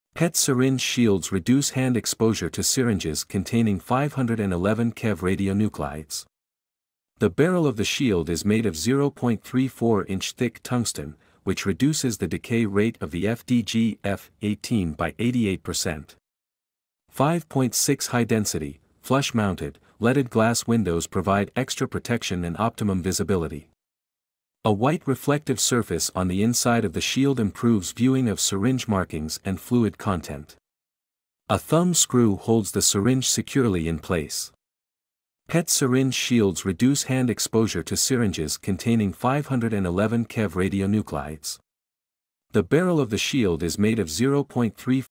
PET Syringe Shield
PET Syringe Shield Tungsten Syringe Shields have their own advantages over other designs. For example, the firmness, stability and safety of Big-Screw are very good, while UNI-Lock is easy to operate, but it is easy for the syringe to fall off accidentally, so medical staff should keep training and review. Big-Screw Tungsten Syringe Shields the whole is tungsten alloy material, with good wear resistance, impact resistance, corrosion resistance, environmental protection and excellent radiation shielding performance. In order to make tungsten alloy materials in the field of nuclear medicine sustainable development and popularization, we should keep pace with The Times, more efforts to improve the production technology.